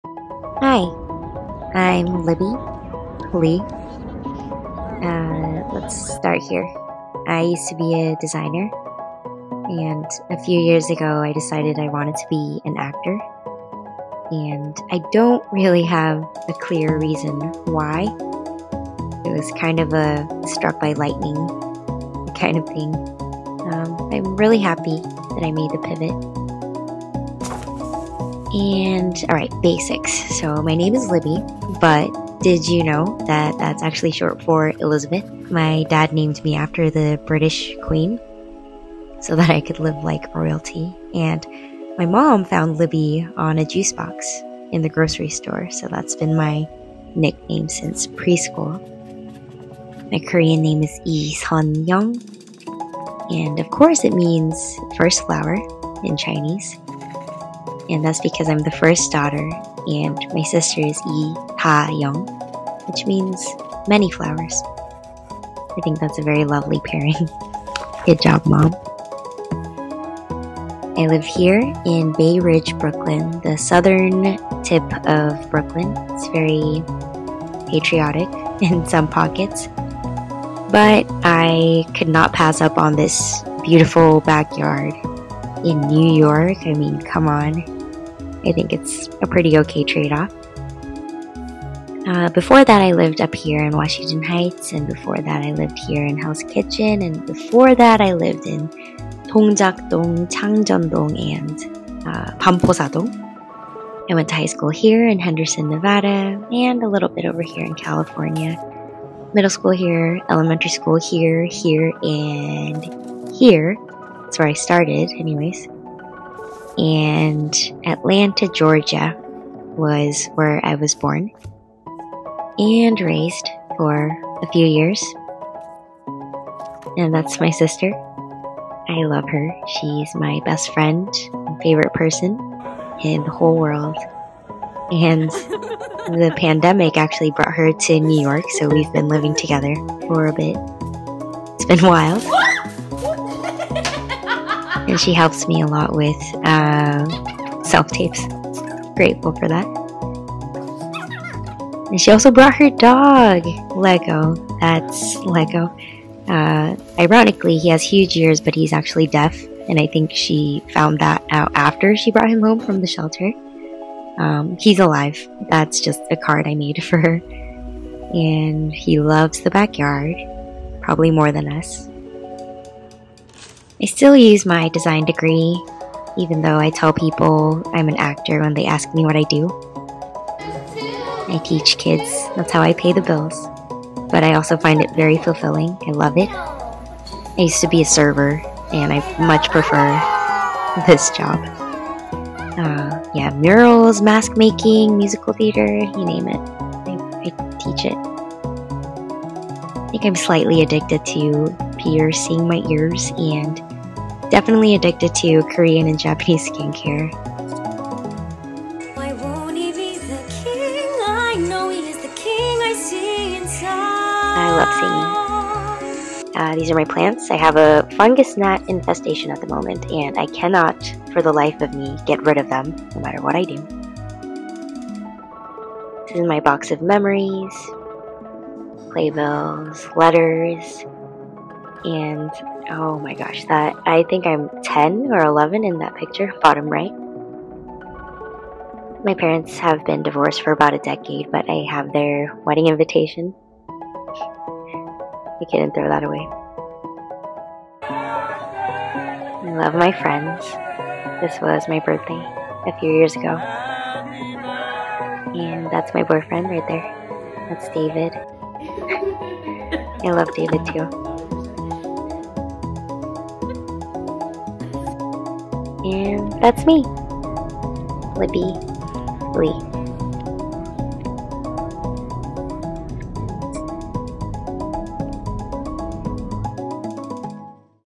It's Me, Libby Lee (audition for ‘Crying in H Mart’)
Audition for 'Crying in H Mart' by Michelle Zauner
A creative video introduction